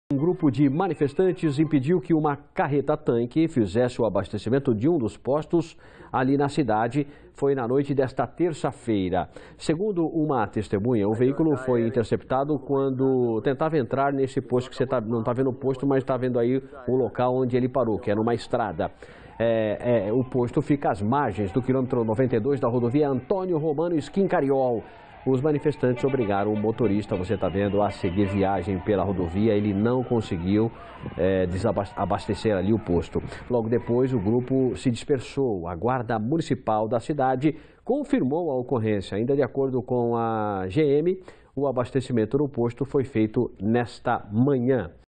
Manifestantes impedem o abastecimento de posto - TV SOROCABA/SBT
E ainda em Cerquilho, um grupo de manifestantes impediu que uma carreta-tanque fizesse o abastecimento de um posto de combustíveis, na noite desta terça-feira.